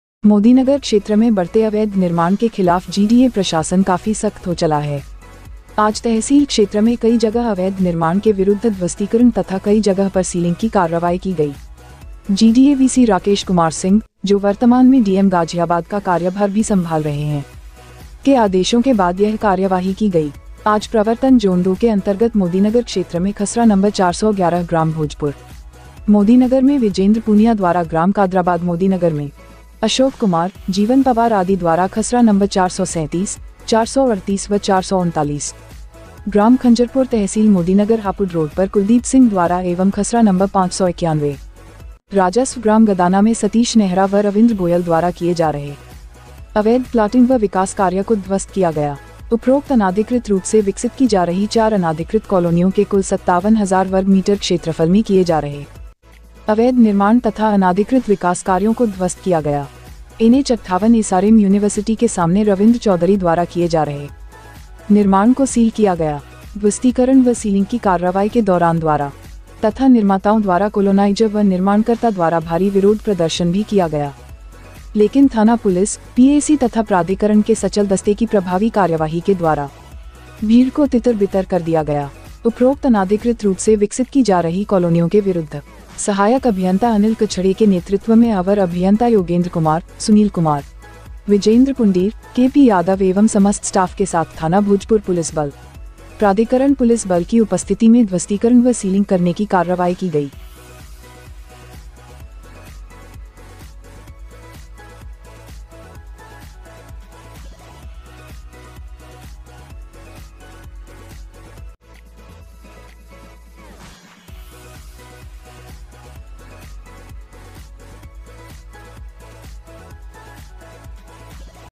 अवैध निर्माण के विरुद्ध चला जीडीए का बुलडोजर @अवैध निर्माण के खिलाफ जीडीए प्रशासन काफी सख्त हो चला
अवैध निर्माण के विरुद्ध चला जीडीए का बुलडोजर

संवाददाता मोदीनगर : *अमित गर्ग*

क्षेत्र में बढ़ते अवैध निर्माण के खिलाफ जीडीए प्रशासन काफी सख्त हो चला है